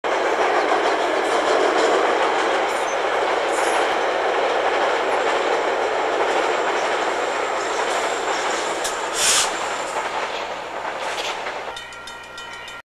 Chicago 2006 36 Metra MP36 Crossing Bridge at Blue Island, IL
Nice to see newer commuter trains